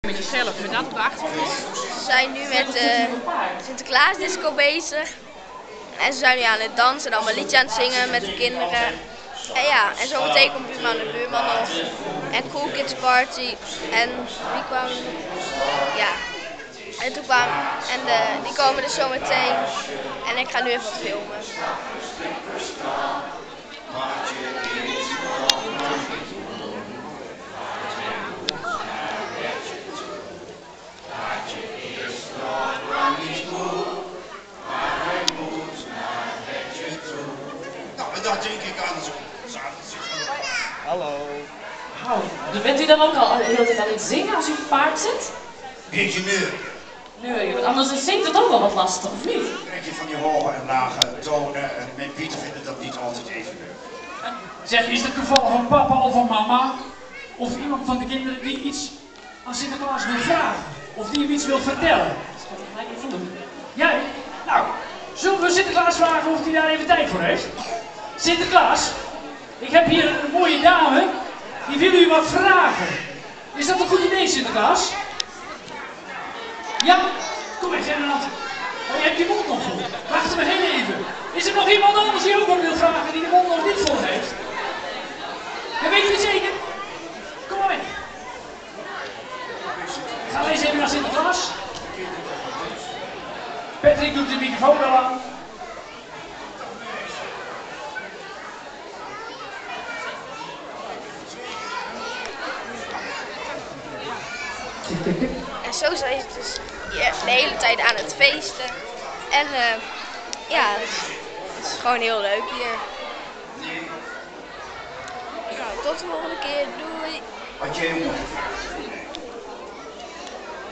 Sint vlogger Robin - Sinterklaashow deel 1
Sinterklaas show deel 1
Robin laat zien hoe hij de show heeft beleeft!